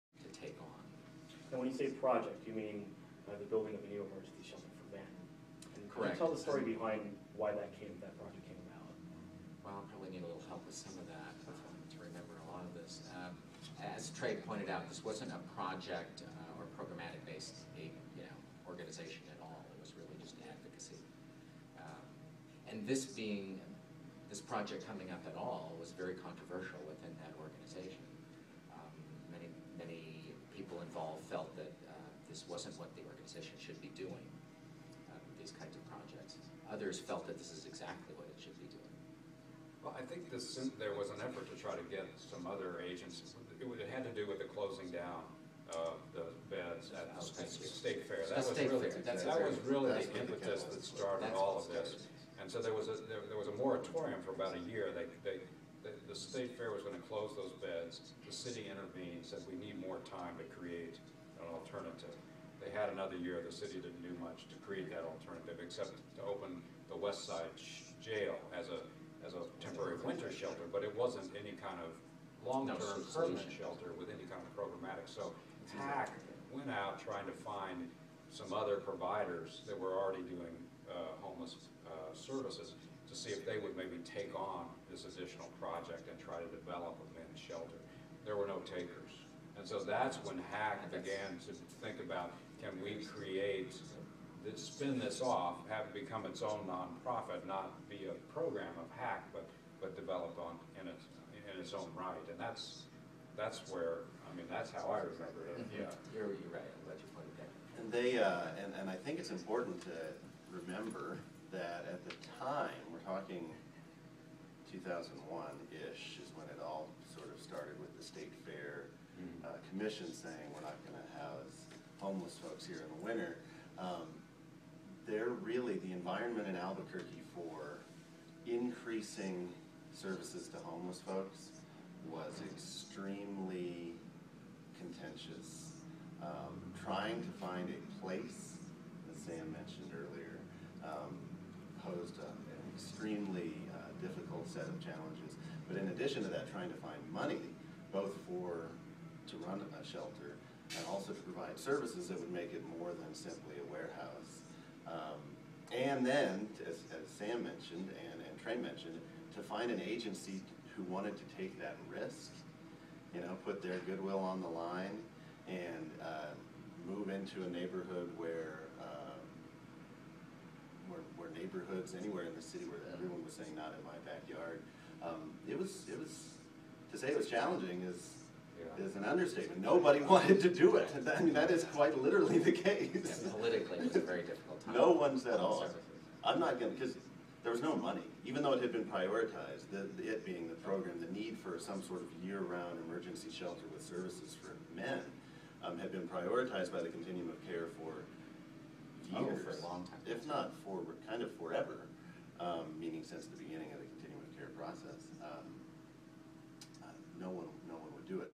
MHP Founding Members Interview - The Homeless Advocacy Coalition (HAC)
Founding members of Metropolitan Homelessness Project discuss the Homeless Advocacy Coalition and the circumstances that led to the beginning of Albuquerque Opportunity Center, an emergency shelter for men.